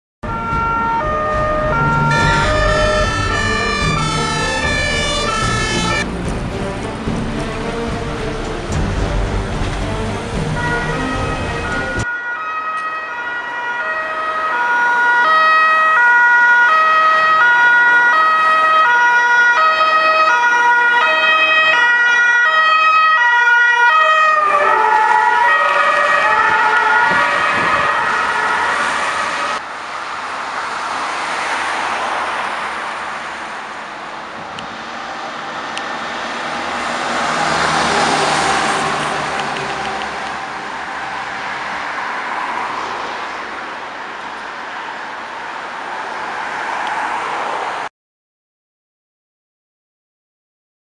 Ambulance NAW Incendie Sauvetage Luxembourg
Ambulance NAW  Incendie Sauvetage Luxembourg avec urgence
. A ALS ambulance of the Luxembourg fire department is seen responding code 3 .
 Copyright video & photo : ThePolicefreak